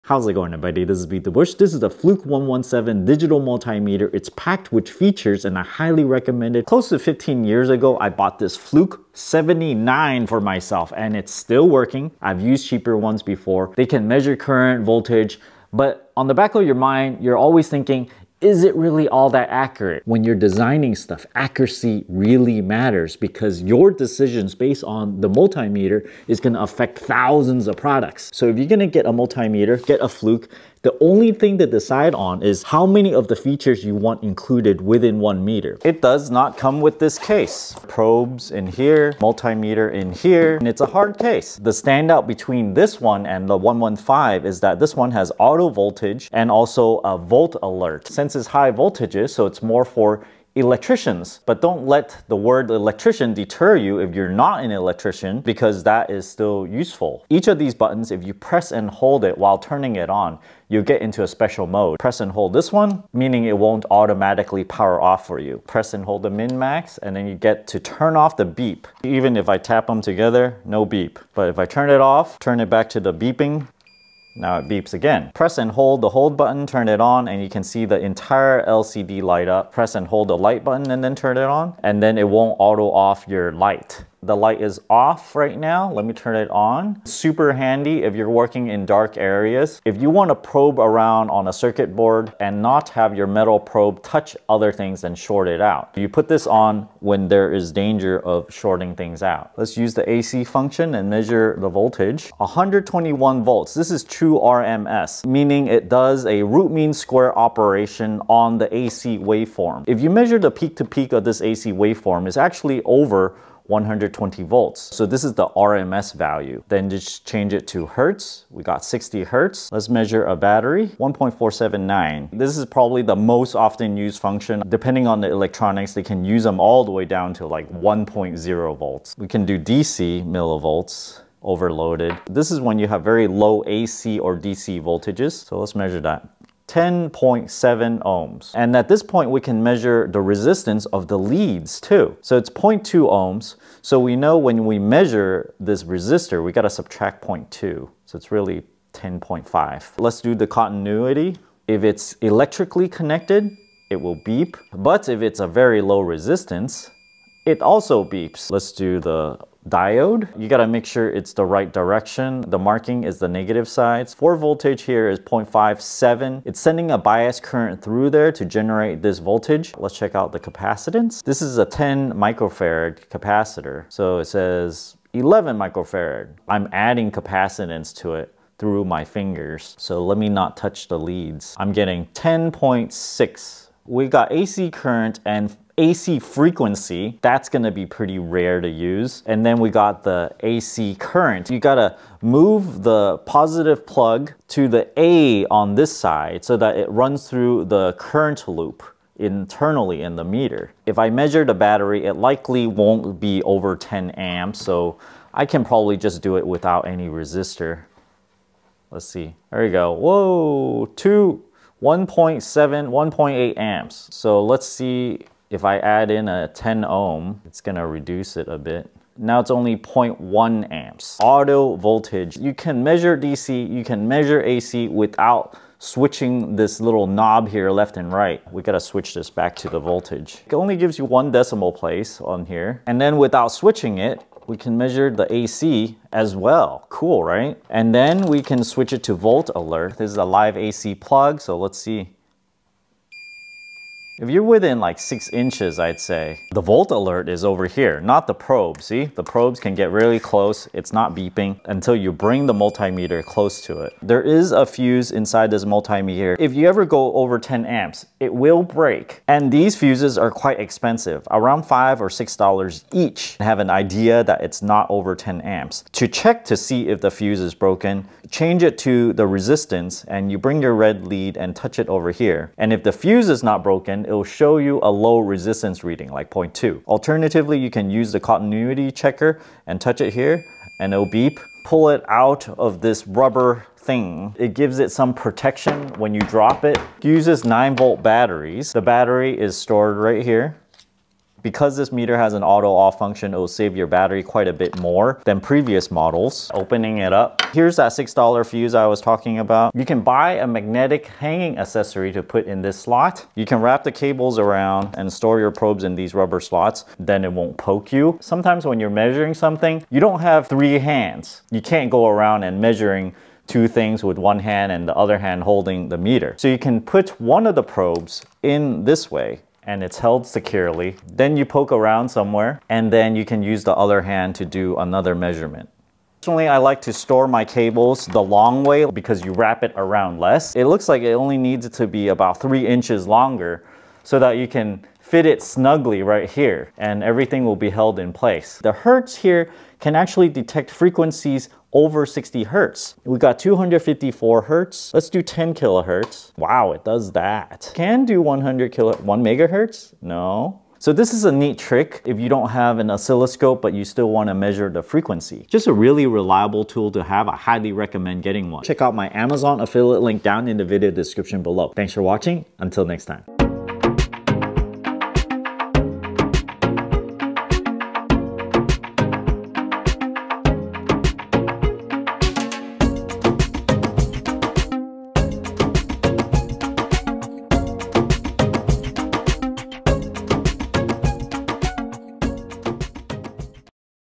How to Use a Digital Multimeter w/ Fluke 117
I tested all the functions in this Fluke 117 digital multimeter including some power-on functions that are not in older models. Flukes are great multimeters, built like a tank, can take a beating, and I've had an older one that is over 15 years old!